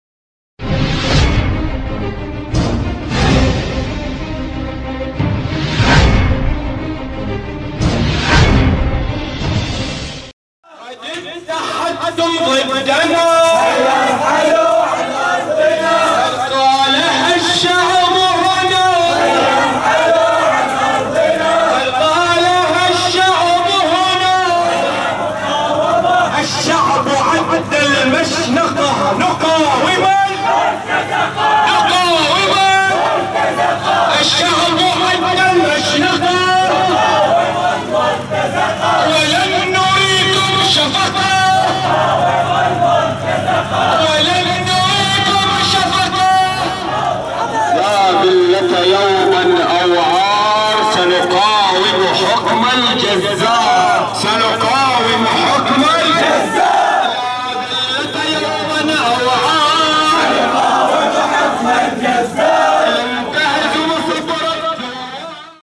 قرية القرية: مسيرة حِصاركم الإرهابي تحت أقدامنا.. تضامناً مع أهل بلدات مثلث الصمود 5-3-2014
قرية القرية: حِصاركم الإرهابي تحت أقدامنا..مسيرة غاضبة تتضامن مع أهلنا الصامدين في بلدات مثلث الصمود وتُعلن دعمها لِخيار المقاومة.
 5-3-2014